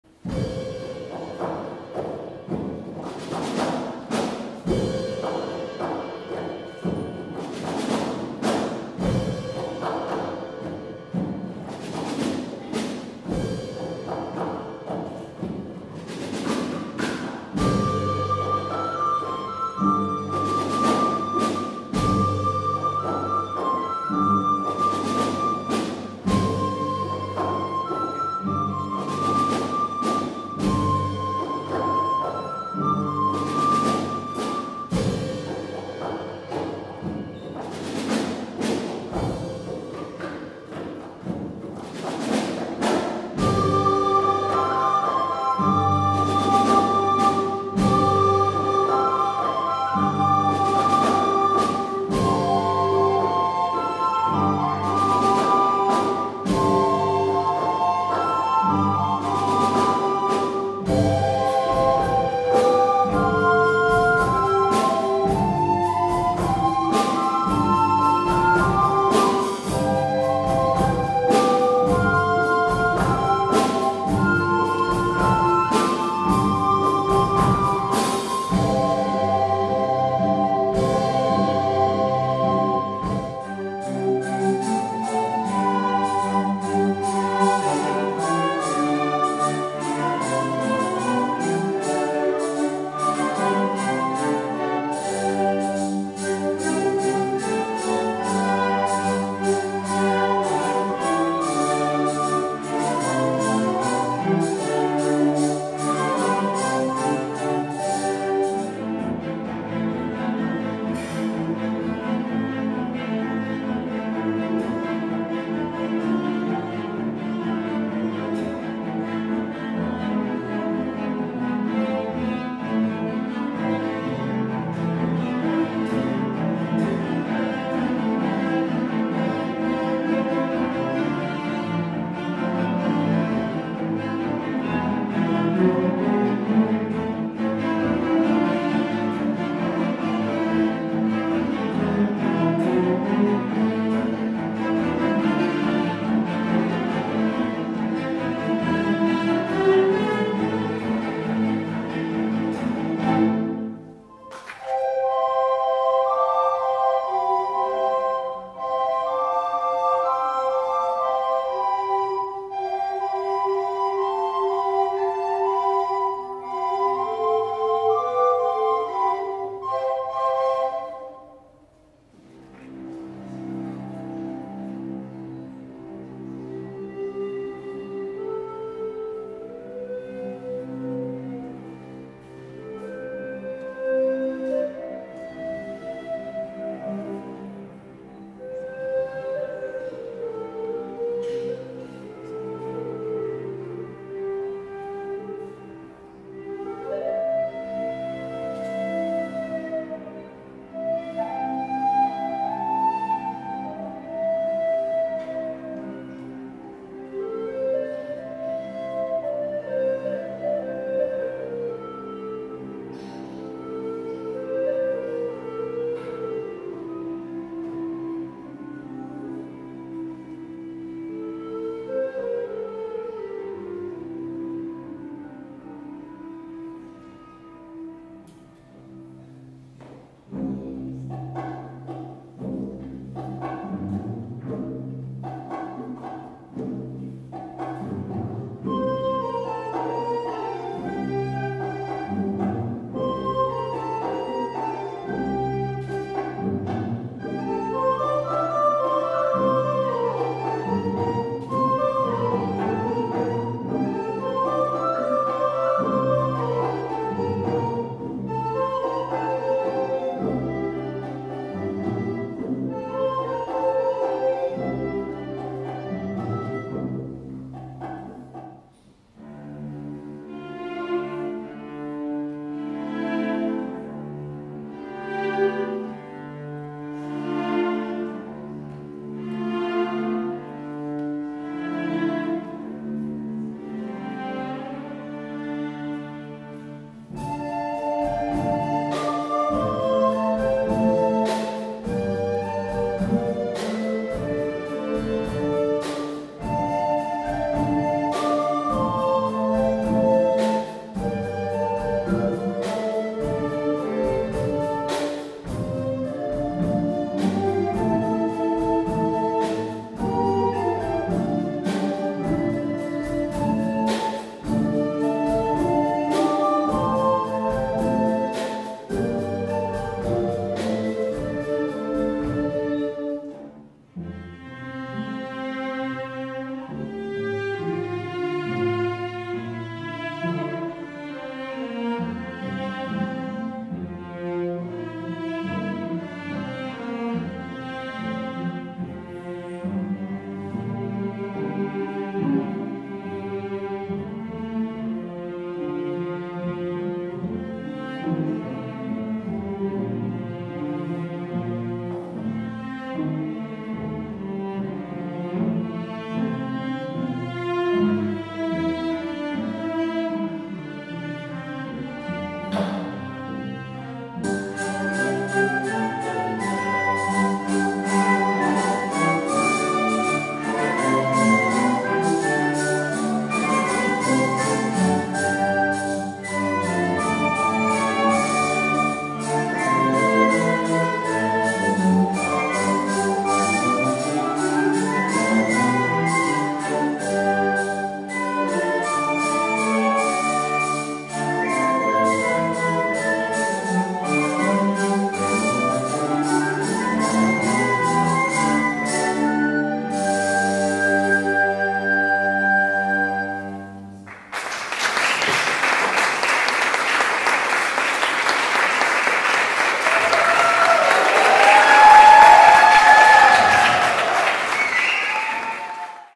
Centre d'interès: La volta al món en 80 dies
L'aula de flauta de bec i el conjunt de violoncels del centre va fer un bonic concert en motiu del centre d'interès. Usant com a fil conductor la novel·la La volta al món en 80 dies de Jules Verne, ens varen fer viatjar per músiques de tot el món.
Enhorabona alumnes i professors implicats!